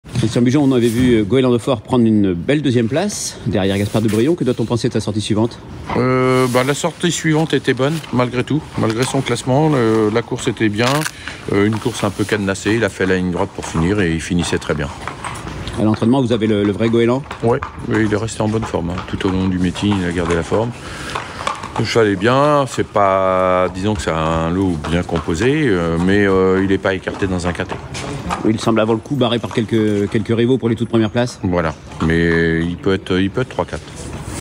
Christian Bigeon, entraîneur de Goéland d'Haufor (Samedi 2 mars à Paris-Vincennes)
Christian Bigeon, entraîneur de Goéland d'Haufor (4) nous parle de ses chances dans le Quinté+, le Prix du Plateau de Gravelle sur l’hippodrome de Paris-Vincennes le samedi 2 mars 2024.